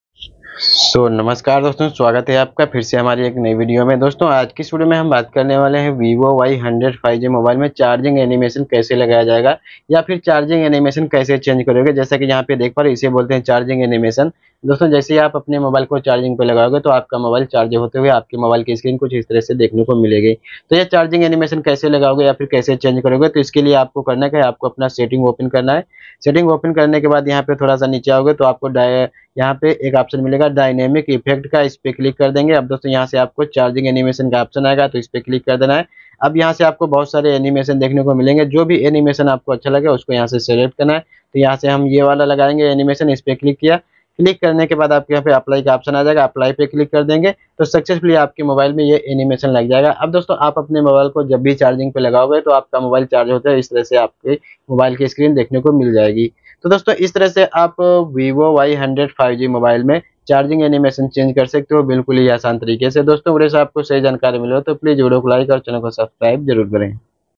Vivo y100 charging animation setting | Vivo y100 me charging animation kaise lagaye
hello friends Aaj ki is video ke madhyam se maine aap logon ko bataya hai ki Vivo y100 mobile me charging animation kaise lagaya jata hai